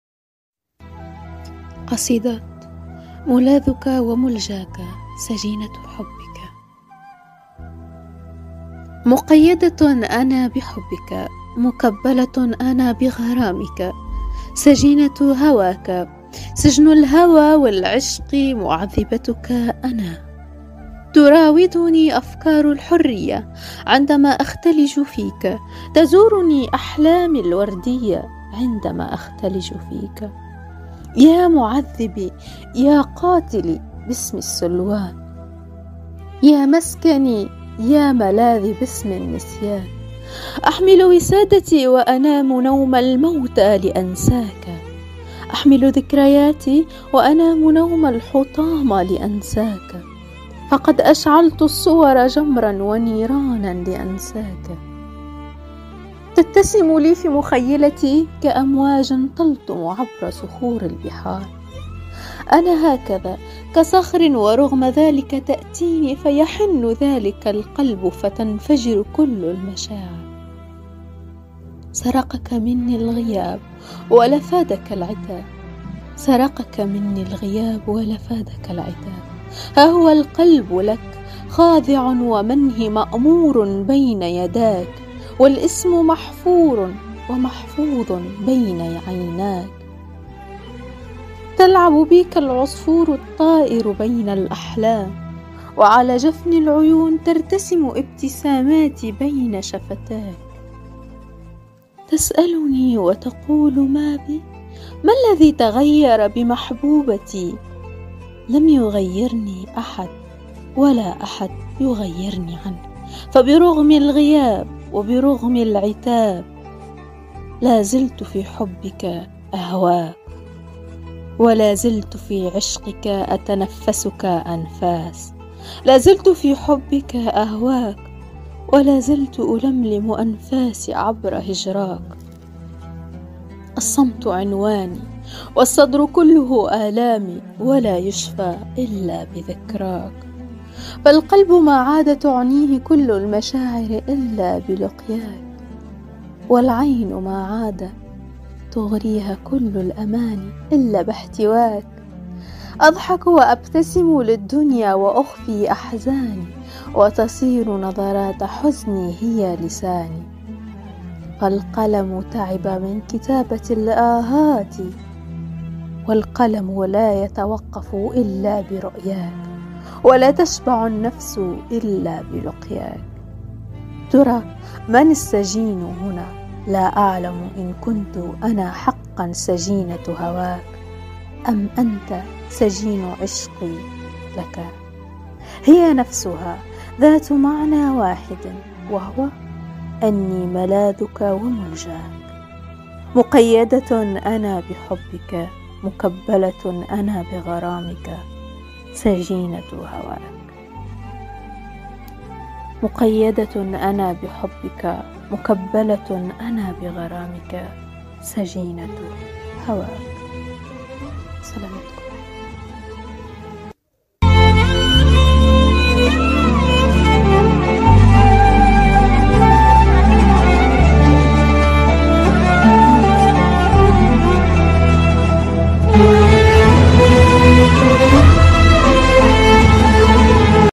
قصيدة ملاذك وملجاك
نعترف بوجود الحب مع من لا يعترف به
بقلم واداء نهلة العجيمي